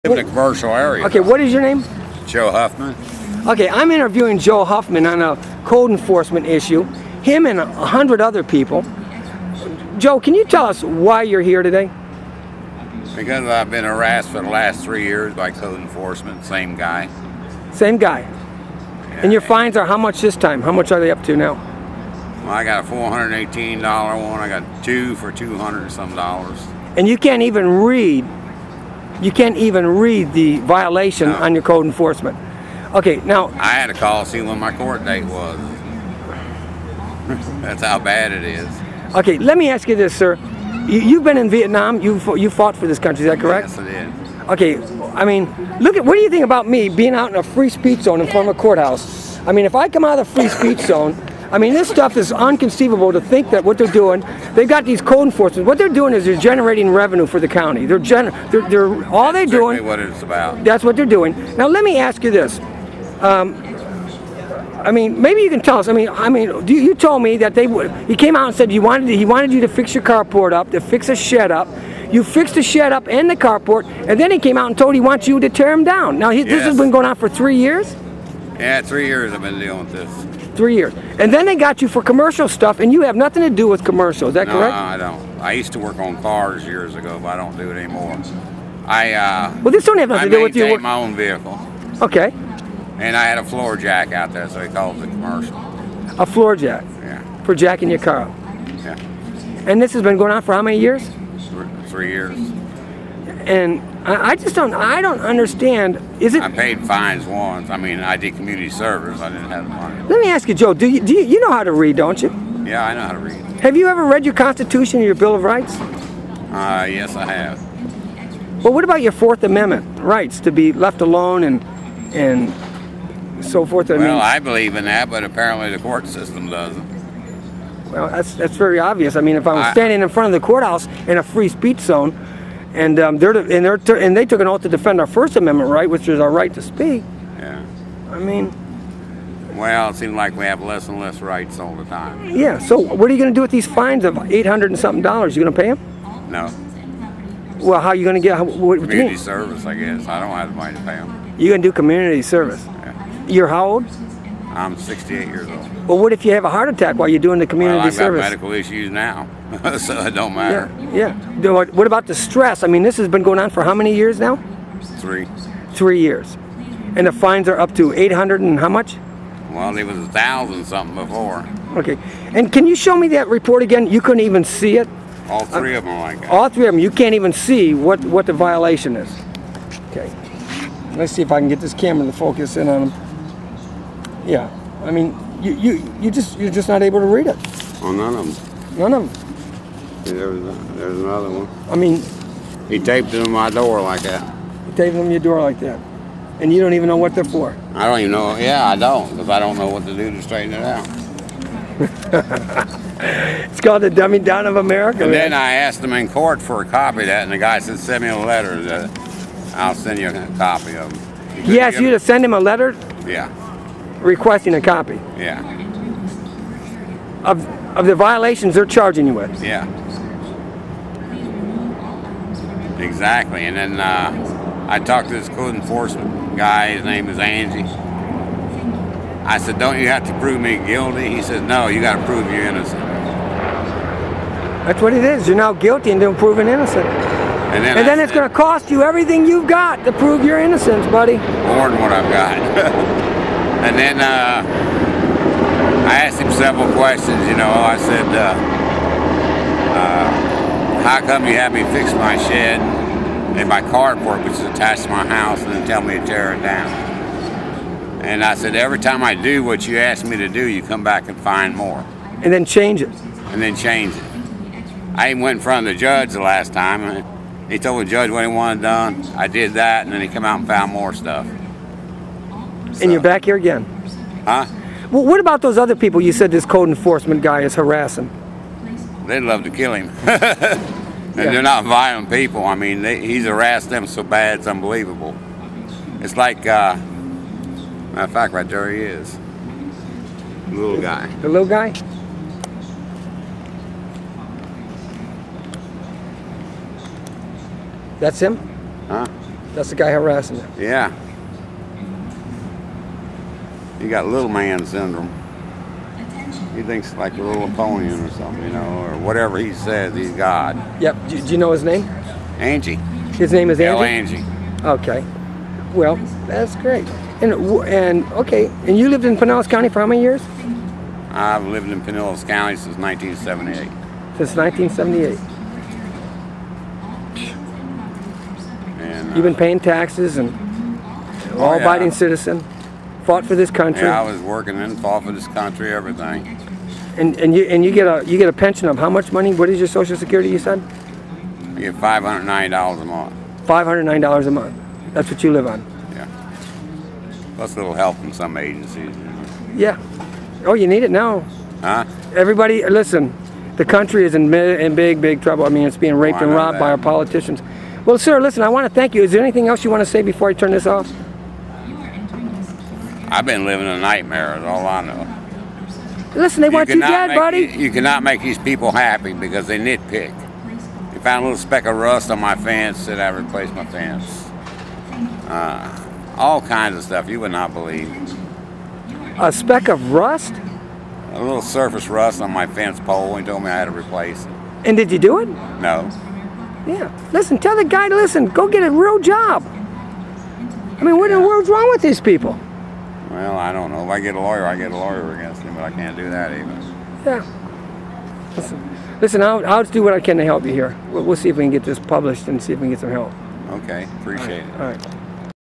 Greg Pound for Sheriff 2012
Code enforcement   generating income for Pinellas County Politicians.  Bleeding the people of Pinellas of their freedom, families, money and every other adjective  you can thinks of.